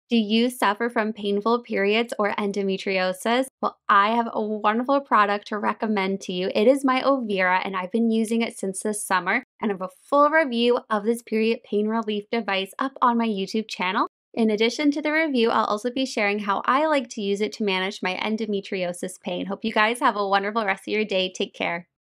Ovira Period Pain Relief Device, great for Endometriosis Pain | Black Friday Sale 30% off!
Do you suffer from painful period cramps or Endometriosis? 💛 I have been testing out my @ovira Period Pain Relief device since the summer and I’m so happy to (finally) share a review on it! I have Endometriosis. After surgery, my pain and symptoms are better, but my first 1-3 days of my period are still quite painful and I still have bloating (AKA Endo Belly, as you can see in this video). I have found my Ovira Period Pain Relief kit SO helpful for my managing my pain. I have always preferred a more natural way to manage and treat pain and the Ovira is just that!

I have a review on my Ovira, how it works and how I like to use it to manage my endometriosis pain. I also talk about if it’s safe for sensitive skin, as well as the pros and cons!

Have you heard of Ovira before? It is currently on sale for 30% off for Black Friday!

Use code EMILY to save 20% off your order!

Use code EMILY to save 22% off the OHM Beads Flower and Nature themed beads!

Shop my bead design, OHM Don't Give Up